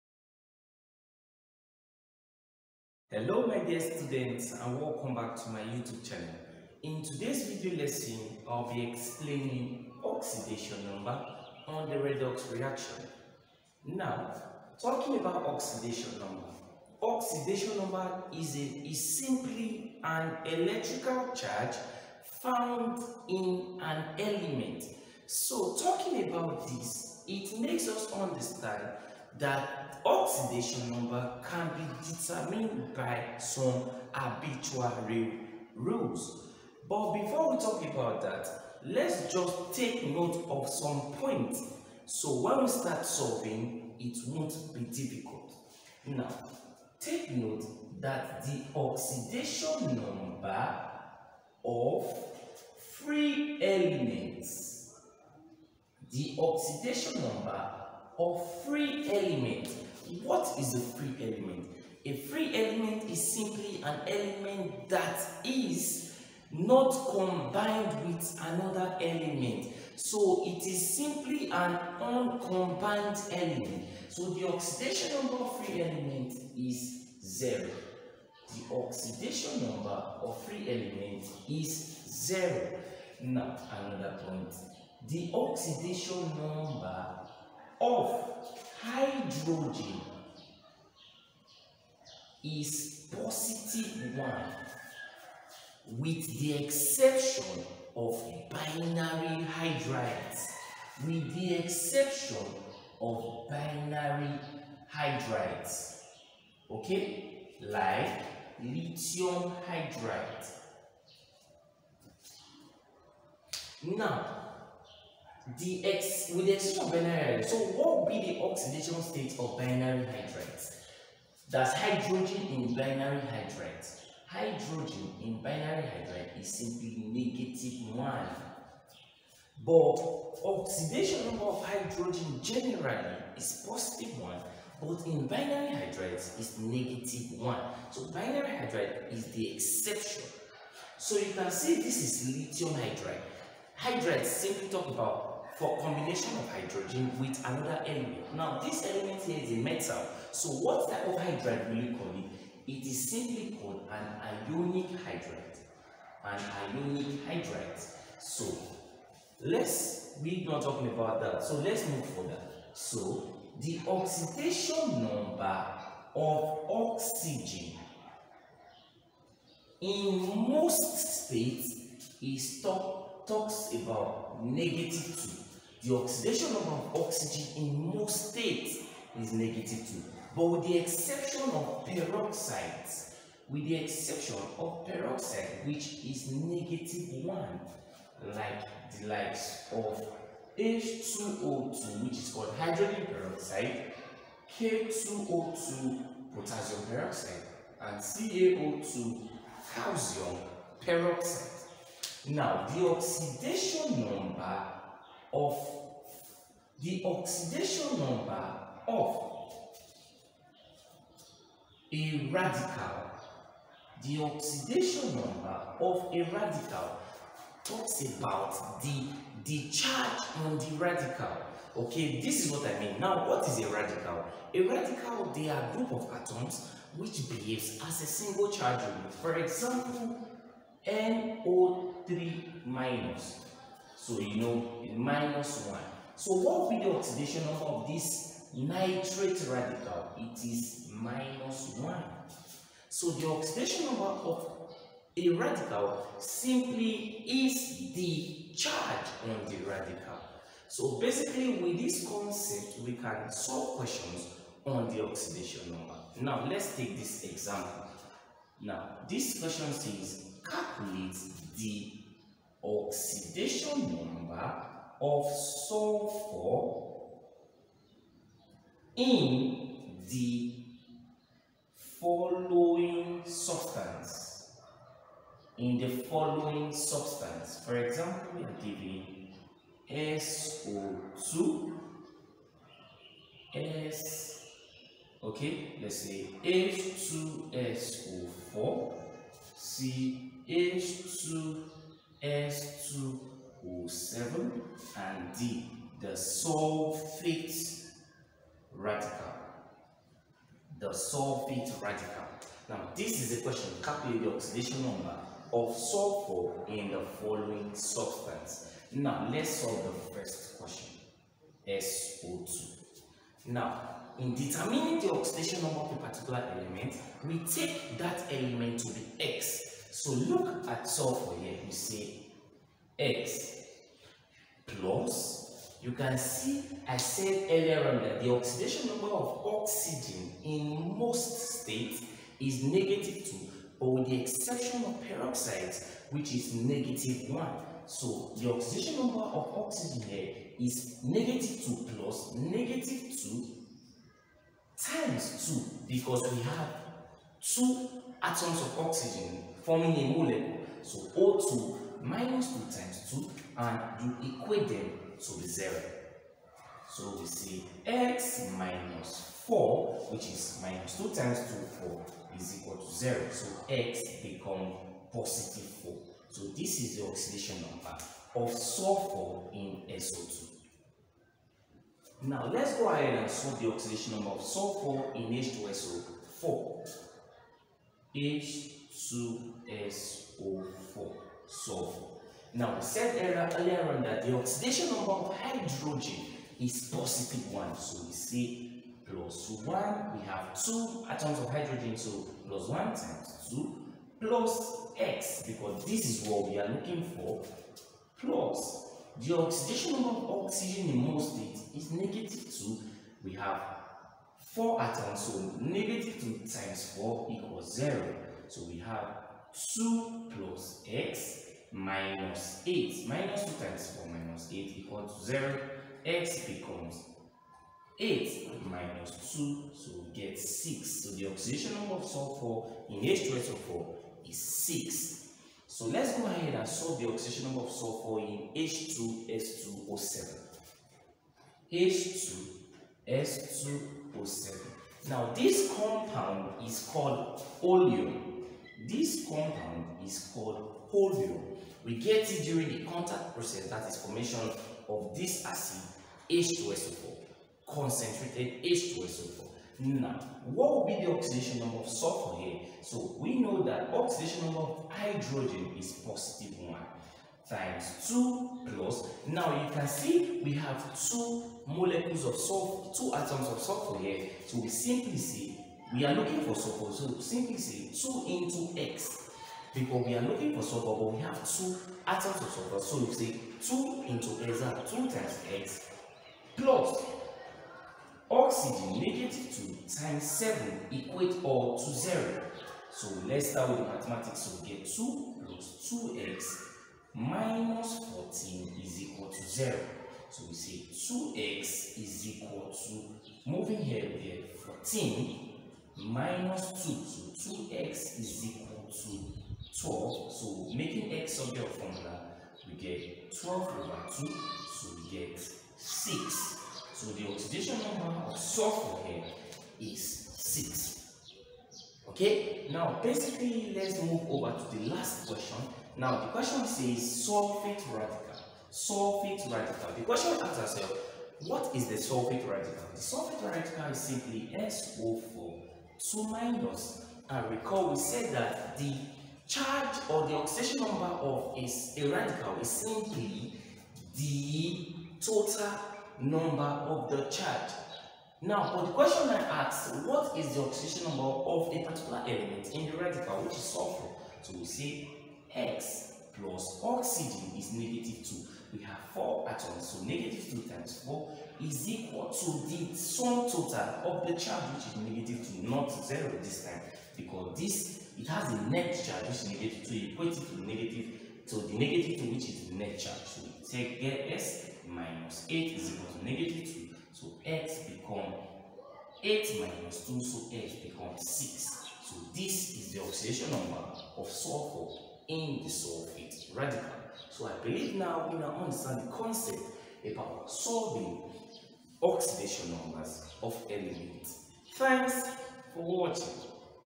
How to Determine Oxidation number of Elements in a Compound
this video lesson teaches on some rules in determination of oxidation number of elements.